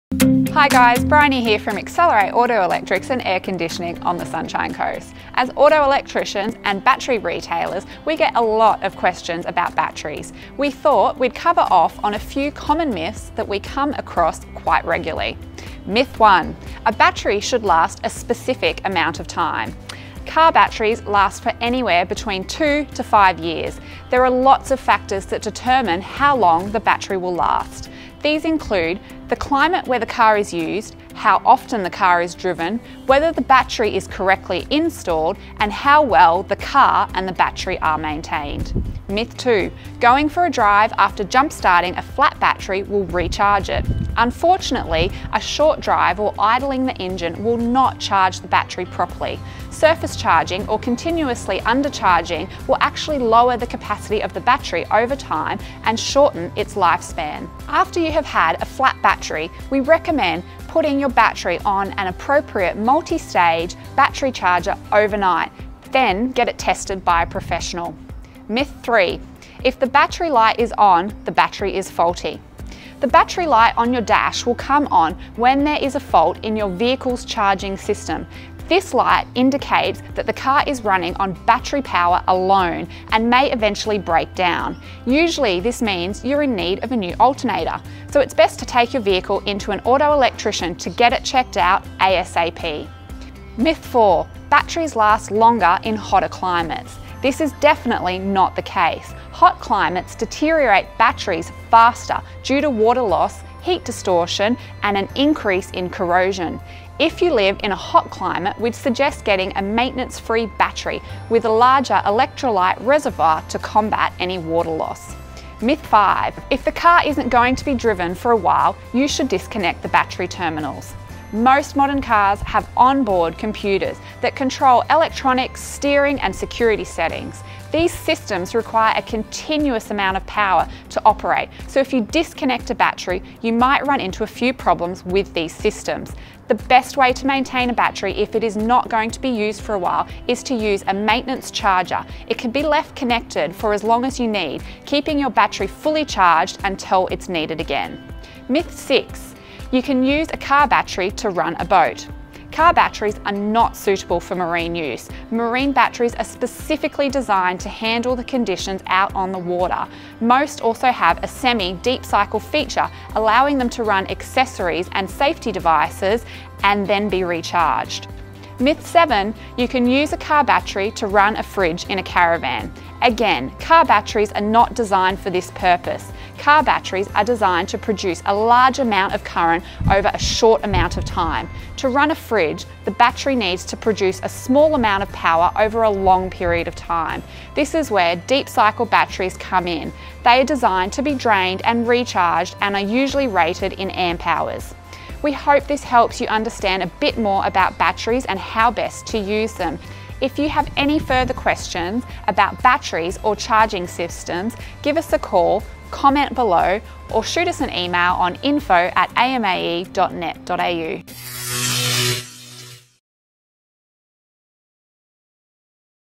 Common Car Battery Myths... BUSTED! | Accelerate Auto Electrics & Air Conditioning
As auto electricians and battery retailers, we get a lot of questions about batteries. We thought we'd cover off on a few common myths that we come across quite regularly.

The myths we are covering in this video are:

Myth 1: A battery should last a specific amount of time.

Myth 2: Going for a drive after jump starting a flat battery will recharge it.

Myth 3: If the battery light is on, the battery if faulty.

Myth 4: Batteries last longer in hotter climates.

Myth 5: If the car isn't going to be driven for a while, you should disconnect the battery terminals.

Myth 6: You can use a car battery to run a boat.

Myth 7: You can use a car battery to run a fridge in a caravan.